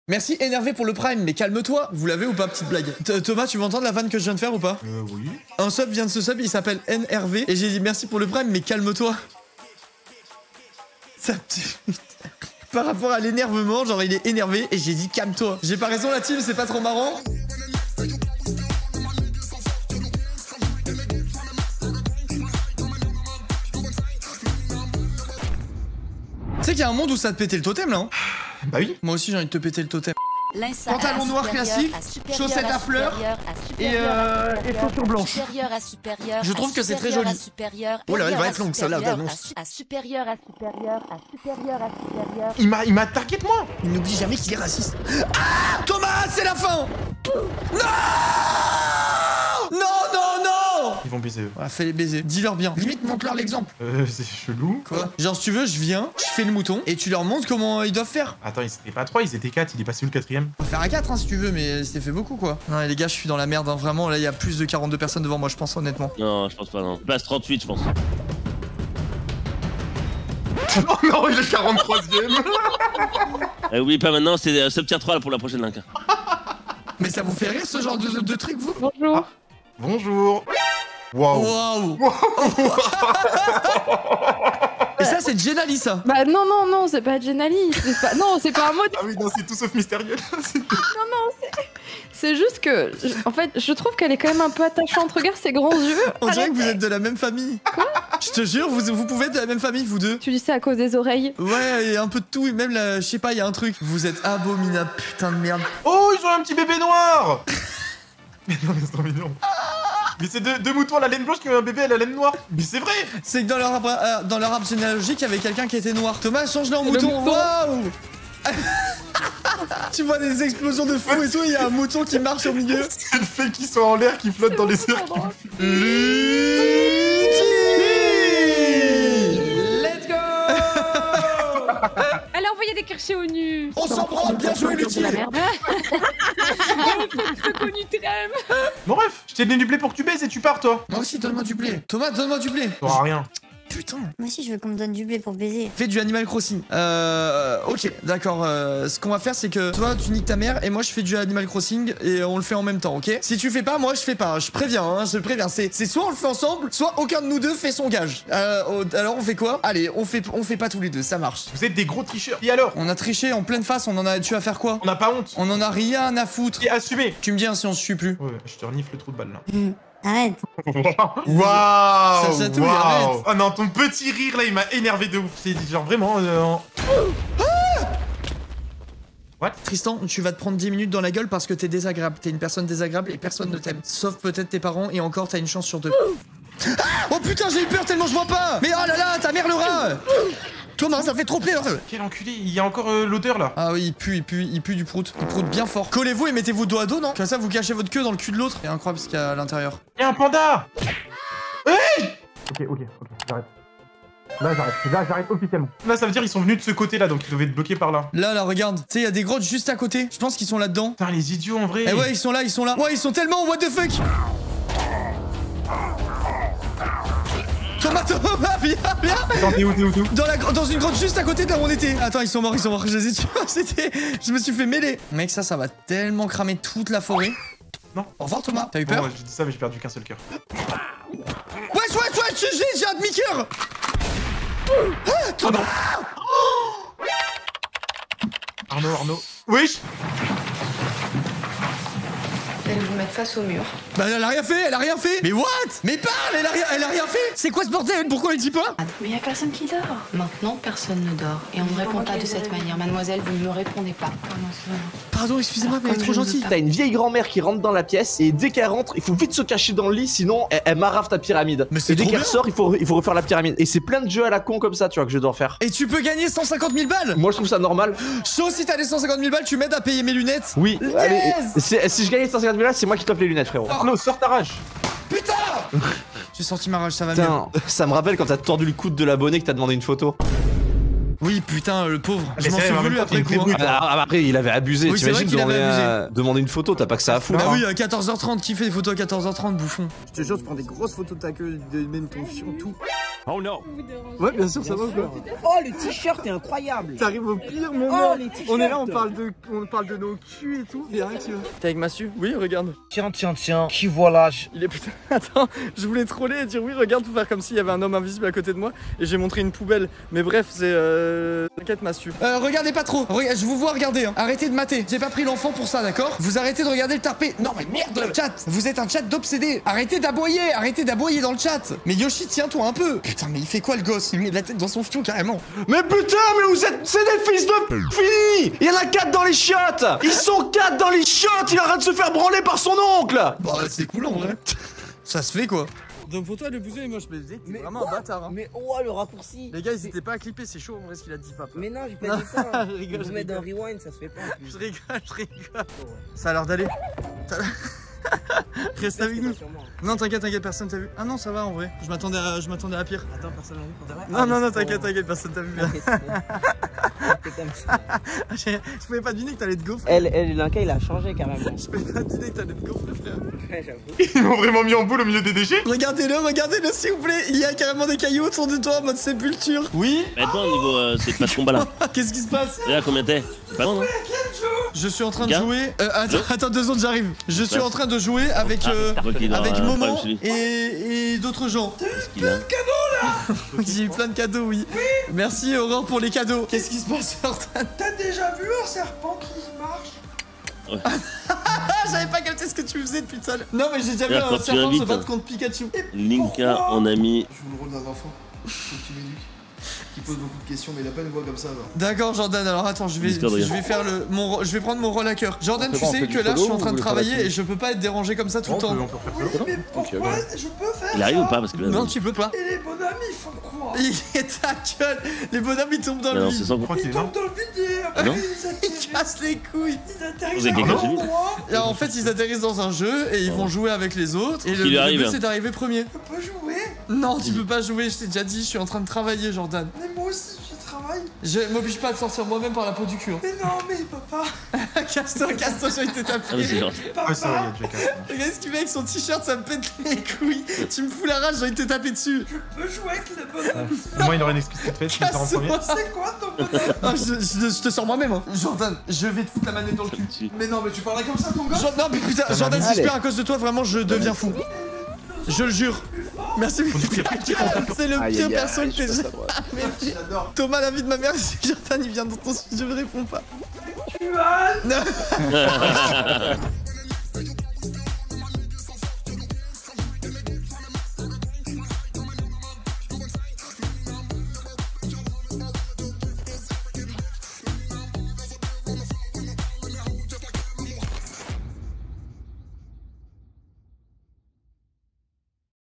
LE FILS DE @Joyca ME REND FOU (Best Of Linca #5)
Le fils de Joyca vient me rendre fou en live ! (Best Of Linca #5)